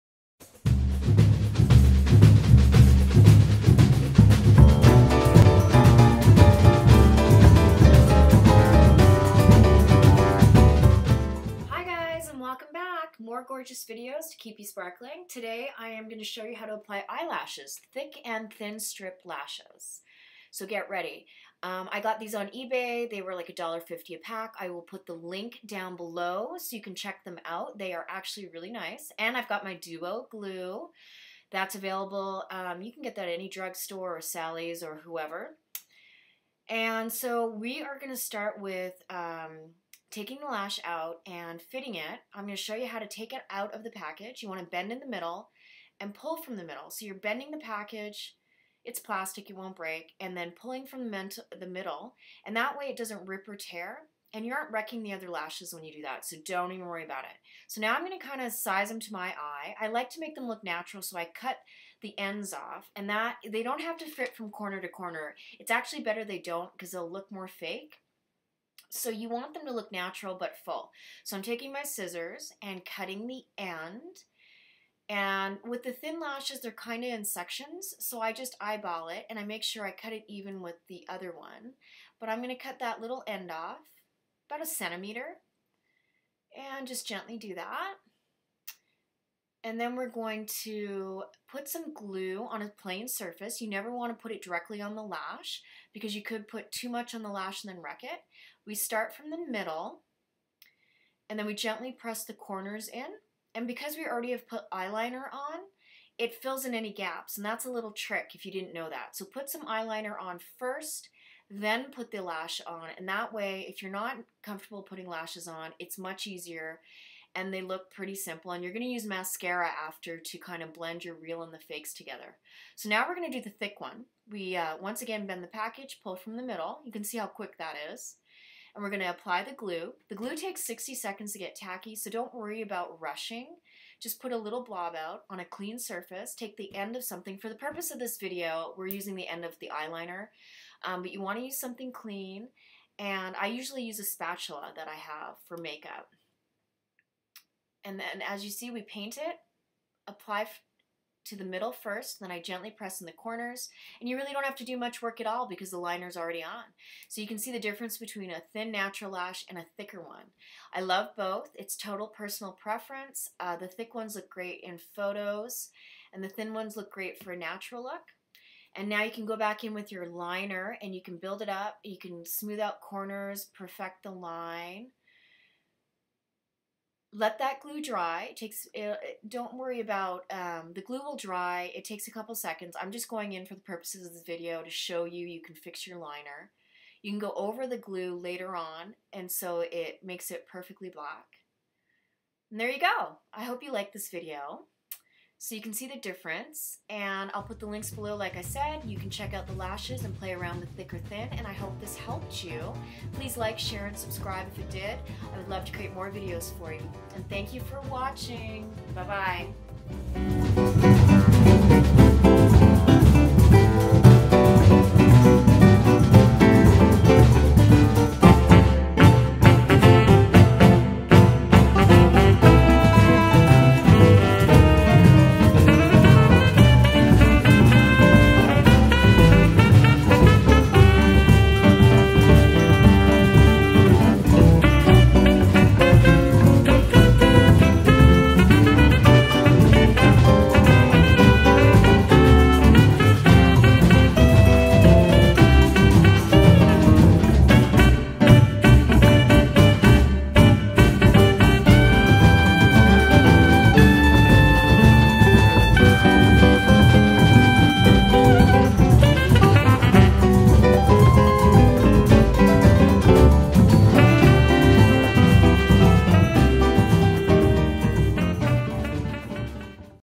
HOW TO APPLY EYELASHES | THIN AND THICK | STRIP | EBAY FIND
HOW TO APPLY EYELASHES | THIN AND THICK | STRIP LASHES | AMAZING EBAY FIND!

Hey pinups and vintage girls more gorgeous videos.  Join and I will show you how to apply natural and dramatic lashes.  Which do you like best? See the amazing gorgeous pack of lashes I found on EBAY for couple dollars!!!

PRODUCT & TOOLS:

EBAY  DRAMATIC LASHES

EBAY NATURAL LASHES

DUO GLUE

DIRTY GLAMOUR MAGAZINE Complimentary for signing up to my mailing list THE RED LIP CONSPIRACY!